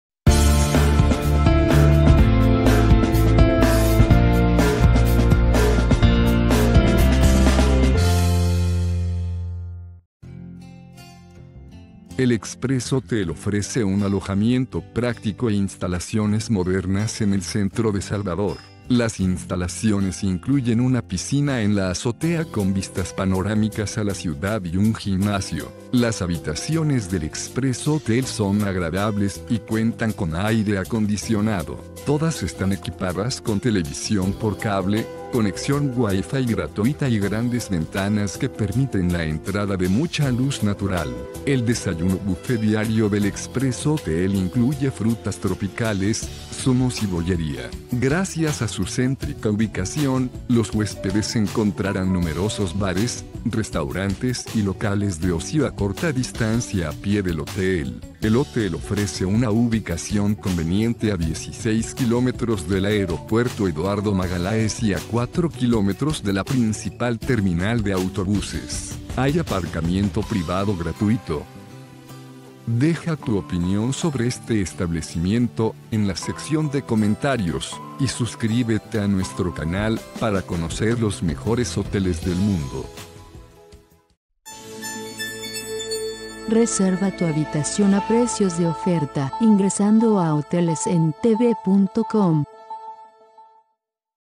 Express Hotel, Salvador
Express Hotel proporciona un servicio 3 estrellas a sus huéspedes. La edificación se instala en la dirección Rua Professor Milton Cayres De Brito, 69 de Salvador, Brasil.